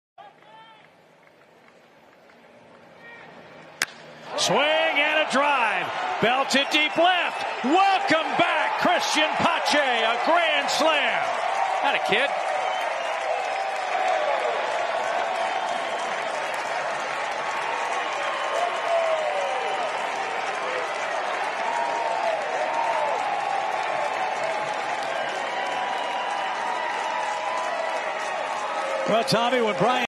Cristian Pache conecta su 1er jonrón de la temporada-21 de la MLB. 🇩🇴
Dominicanos en Grandes ligas. 🇩🇴
Vean los otros vídeos.
Deja tu like.